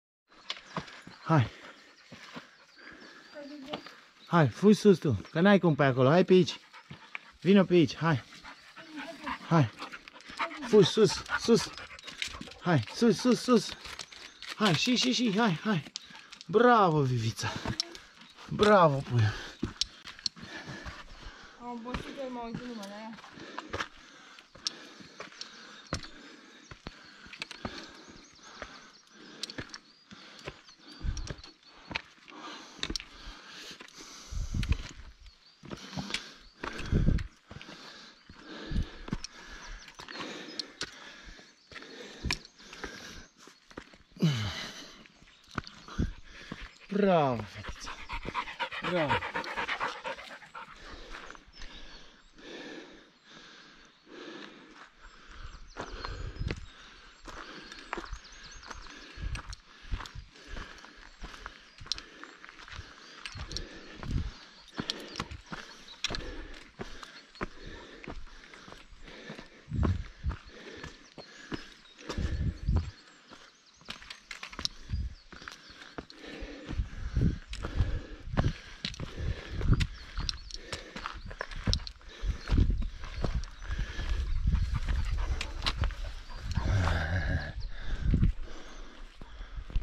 Jepii Mici Muntii Bucegi 22 07 20 Hiking in Transilvania Silviu Hrusca
O filmare cu urcarea noastra pe Jepii Mici din tura 22 iulie Busteni- Jepii Mici- cabana Caraiman- Babele / Sfinx -varful Omu- refugiul Tiganesti- poarta Bran- castelul Bran timp aprox : 9 ore